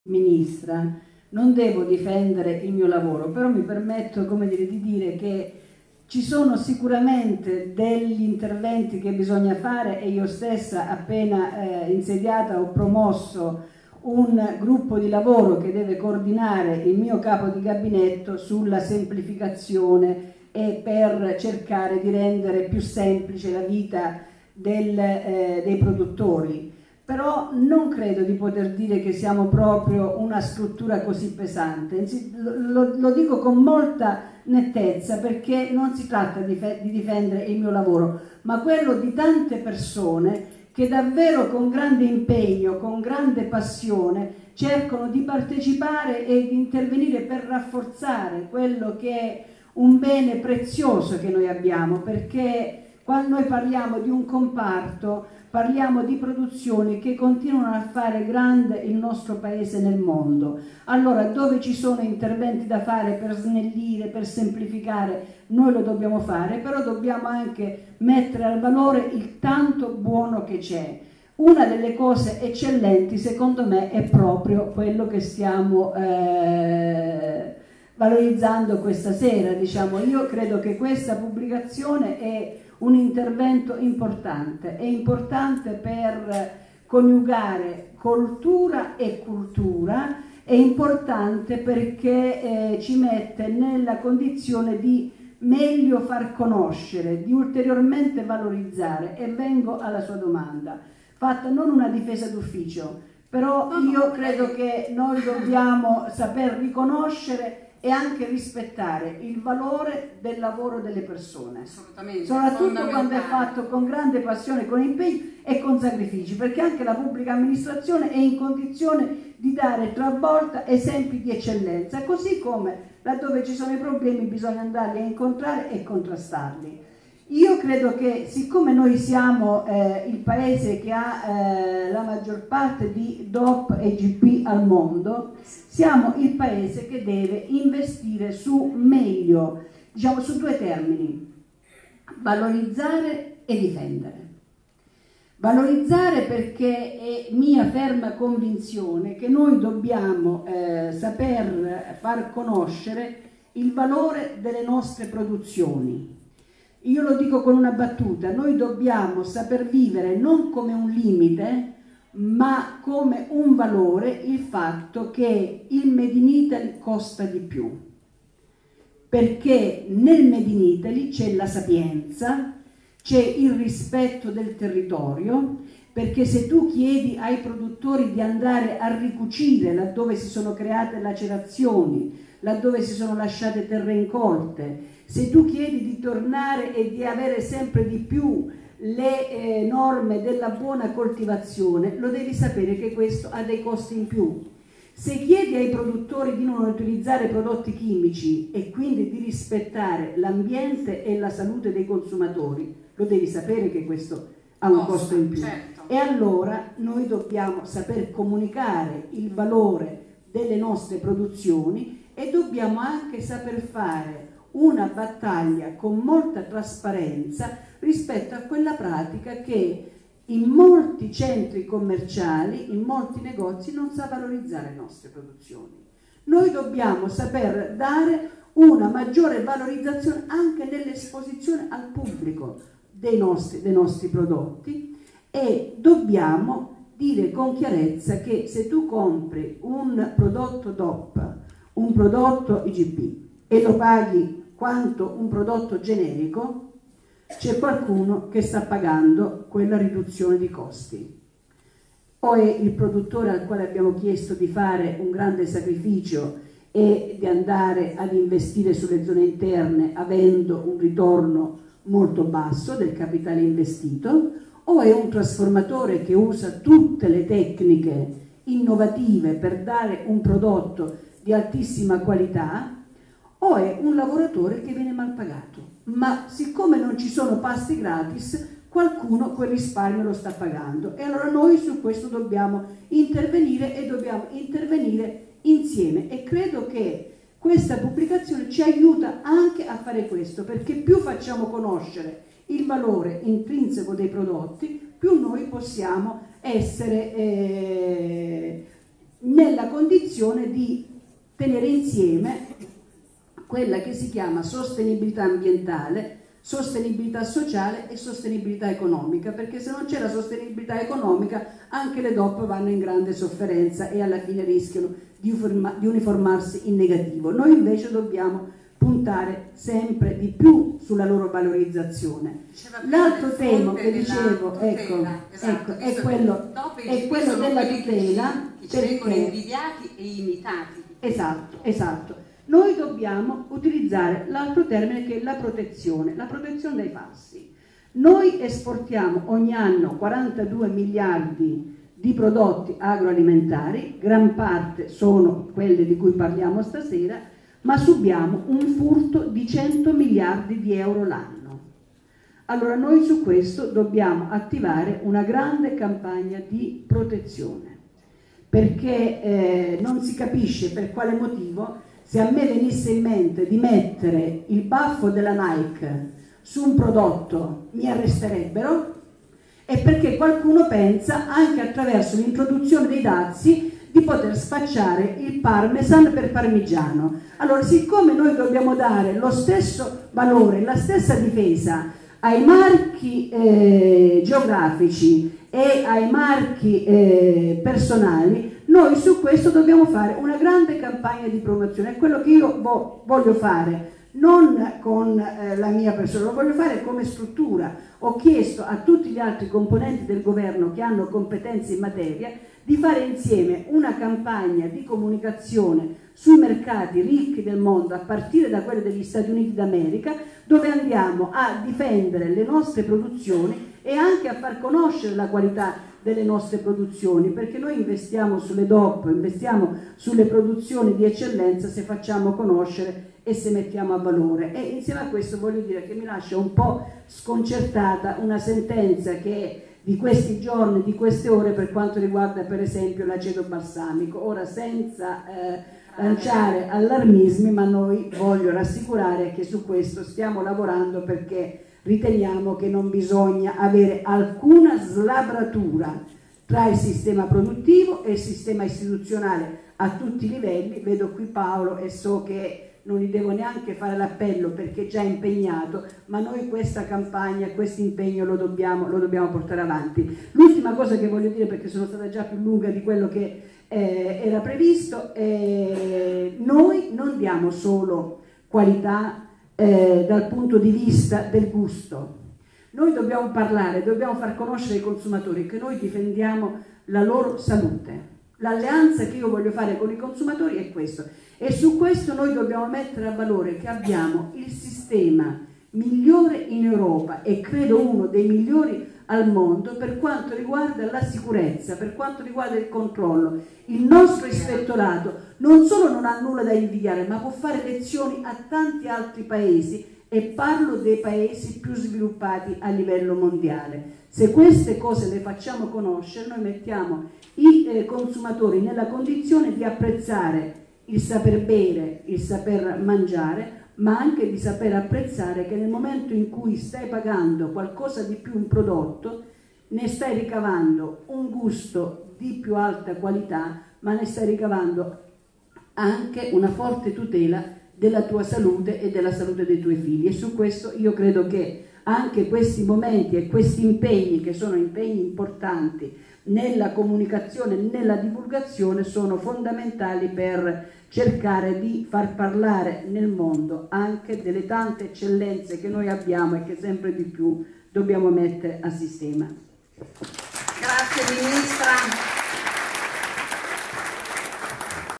Intervento alla presentazione dell'Atlante Qualivita 2020
Le nostre produzioni agroalimentari fanno grande l'Italia nel mondo.
L'ho ribadito oggi alla presentazione dell'Atlante Qualivita 2020, un volume importante perché intreccia coltura e cultura e ci permette di far conoscere il nostro made in Italy. Siamo il Paese con il maggior numero di DOP e IGP al mondo. Dobbiamo valorizzare questa nostra grande ricchezza, agendo su due fronti: la valorizzazione e la difesa.
Dobbiamo prima di tutto valorizzare le nostre produzioni. Il costo del made in italy non è un limite: è un valore. Nel made in Italy c'è sapienza, rispetto del territorio, qualità e salute. Per questo il prodotto costa di più! E
più riusciremo a far conoscere il valore dei nostri prodotti, più saremo in grado di realizzare una sostenibilità ambientale, sociale ed economica.
Per quanto riguarda la difesa dei nostri prodotti, dobbiamo puntare su campagne di tutela, valorizzazione e promozione della qualità del made in Italy nel mondo. L'esportazione dei nostri prodotti alimentari vale 42 miliardi di euro. Il mercato dei falsi e delle contraffazioni vale 100 mld. Un vero e proprio furto di identità che dobbiamo contrastare con tutte le nostre forze.